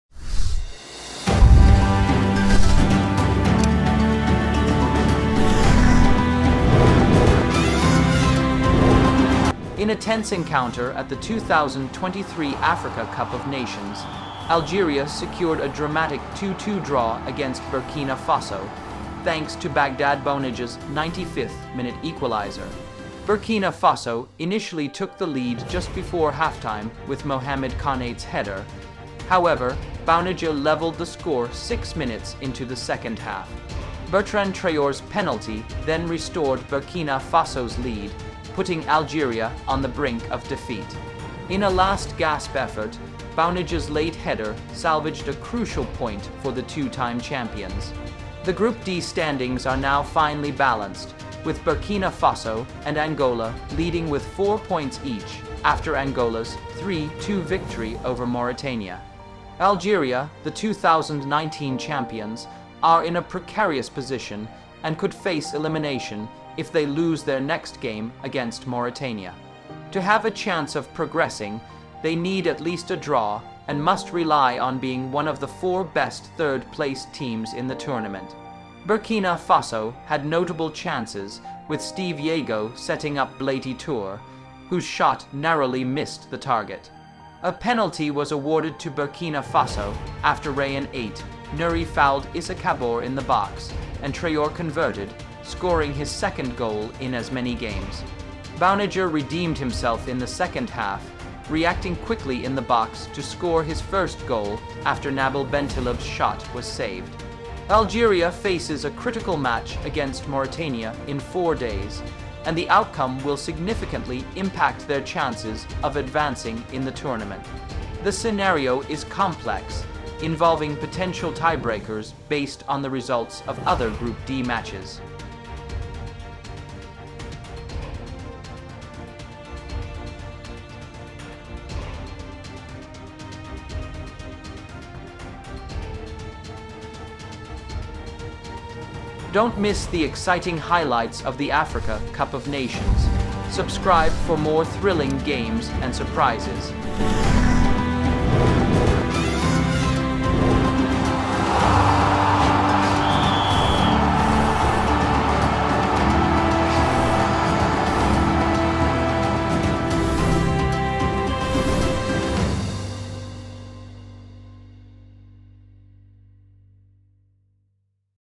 Algeria vs Burkina Faso | Group D | CAF Africa Cup of Nations
In a tense 2023 Africa Cup of Nations clash, Algeria and Burkina Faso played to a dramatic 2-2 draw. Burkina Faso initially led with Mohamed Konate's first-half header, but Baghdad Bounedjah leveled six minutes into the second half. Bertrand Traore's penalty restored Burkina Faso's lead, seemingly securing the win. However, Bounedjah's late 95th-minute header salvaged a crucial point for Algeria. The Group D standings now see Burkina Faso and Angola at the top with four points each, while defending champions Algeria face a precarious situation, needing at least a draw against Mauritania and relying on favorable outcomes in other group matches to advance.